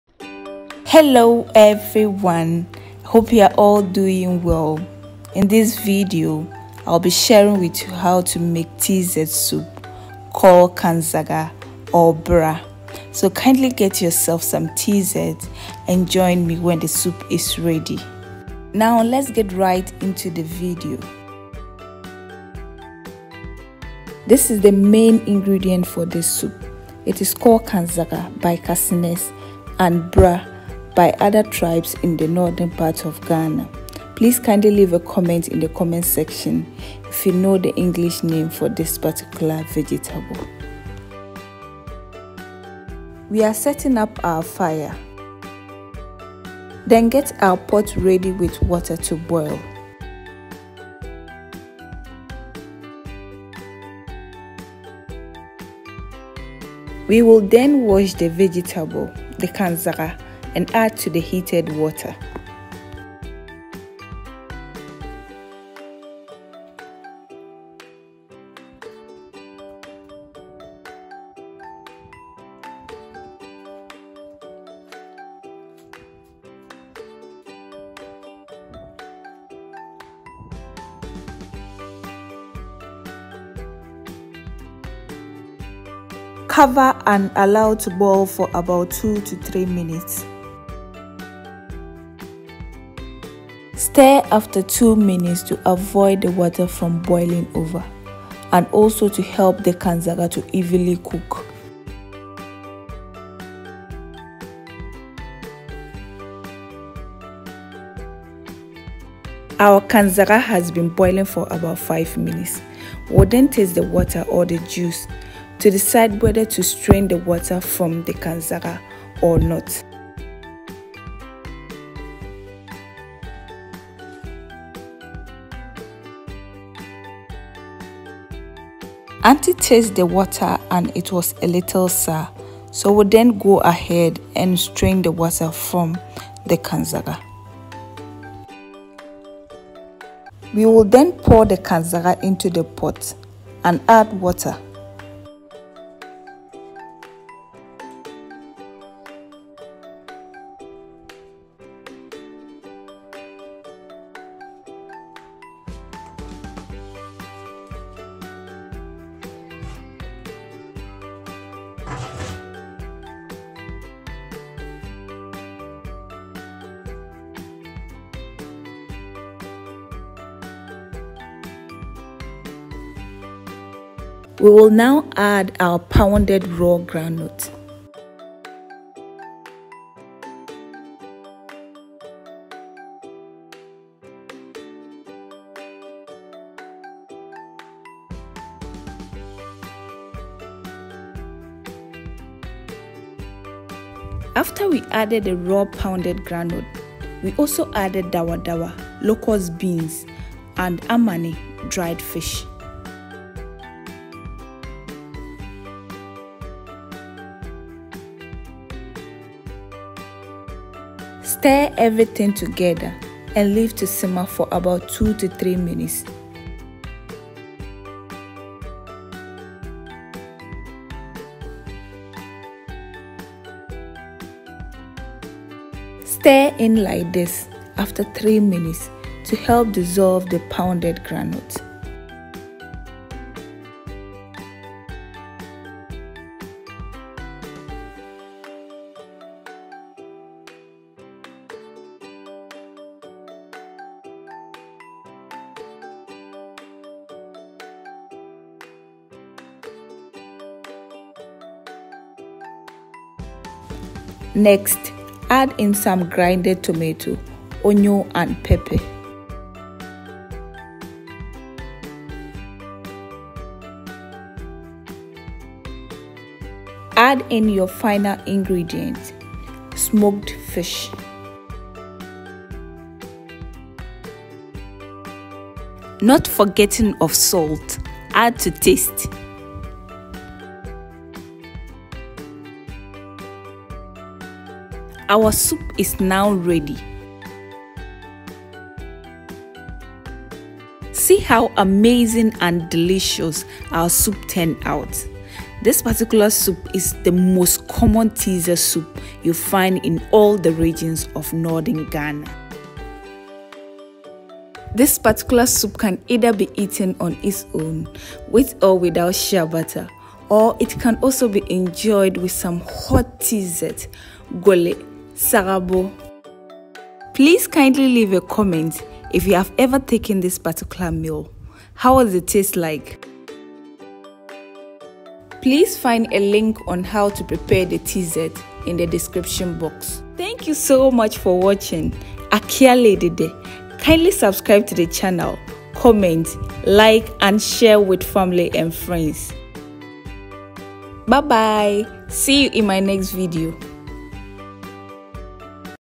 How to prepare the most common Northern Ghana TZ soup/ Bra soup/ Bito/ Kanzaga/ Navrongo Style
This is one of the most common Tz soup in northern Ghana. It is one of the soup you will find any day, any time in all the regions in northern Ghana as a Tz(Tuo zaafi). It is made from a unique vegetable known to all these regions, raw pounded groundnut and any protein of one’s preference and local spices like Dawadawa.
Ingredients :
Kanzaga/ bra leaves ( the vegetable)
Raw groundnut
Fish
Herrings
Pepper
Tomato
Water
Dawadawa/ locust beans
Salt

Below is the link to the Tuo zaafi (TZ)